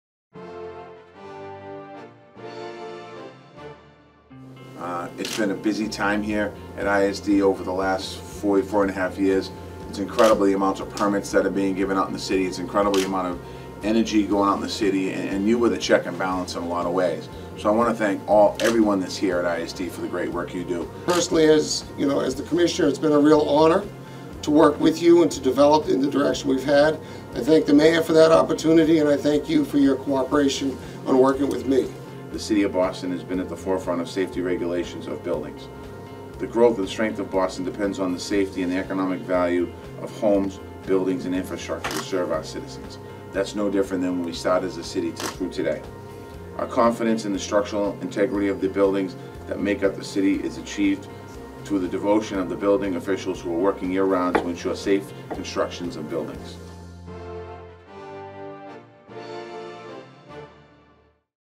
Building Safety Month Proclamation
Mayor Walsh visits the Inspectional Services Department headquarters in Dorchester to thank employees for their work during the city's building boom. The mayor is also joined by Commissioner William "Buddy" Christopher to read a proclamation in honor of building safety month.